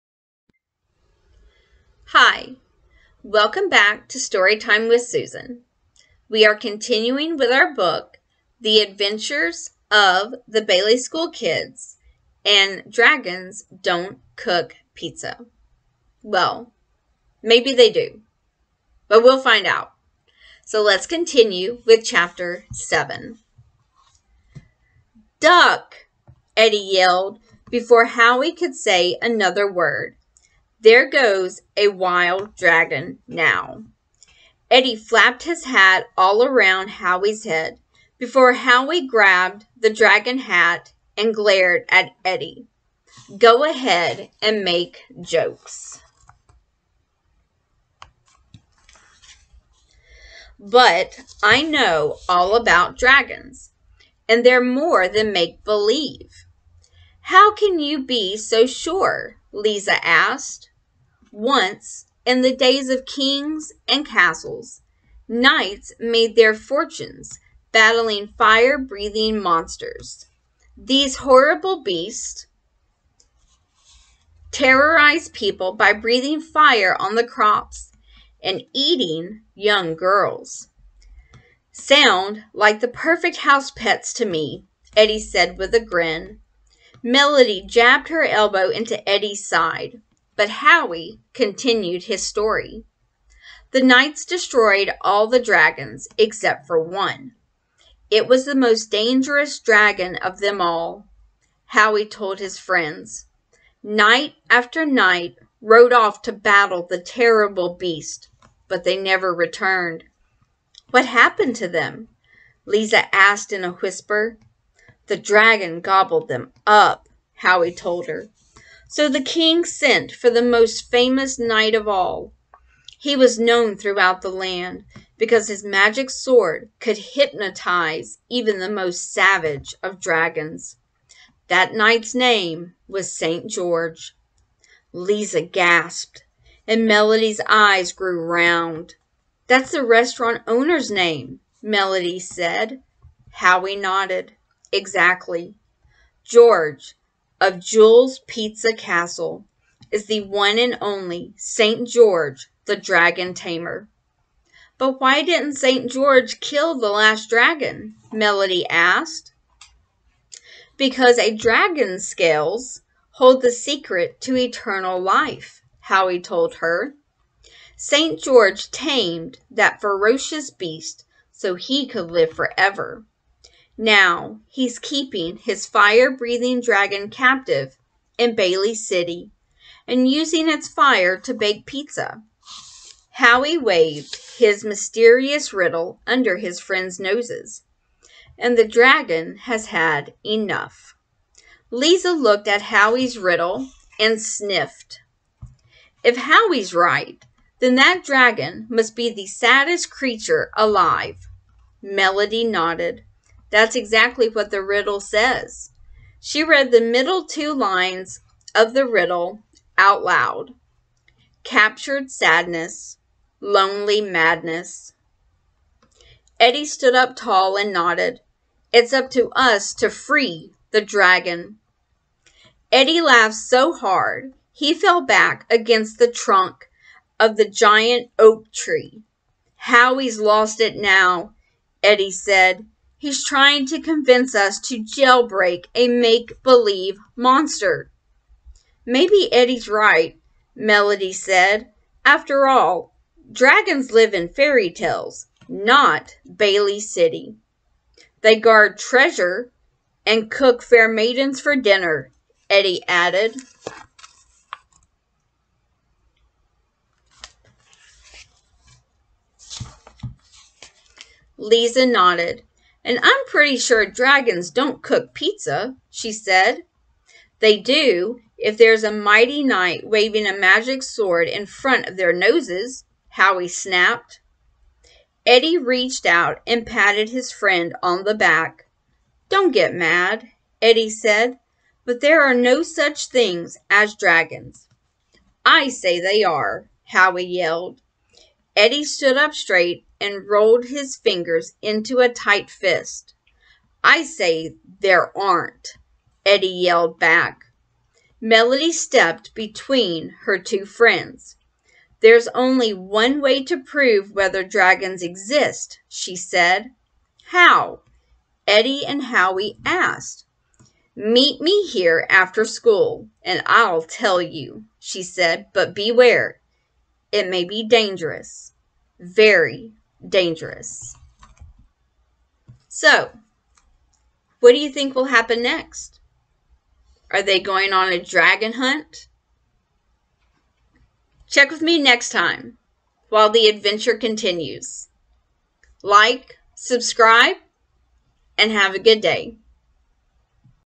Bailey School Kids Dragons Don't Cook Pizza Chapter 7
Storytime with Susan presents The Adventures of the Bailey School Kids Dragons Don't Cook Pizza
Written by Dadey Debbie and Jones Marcia, Thornton
Chapter 7
Request Books Read Aloud by Commenting on this Video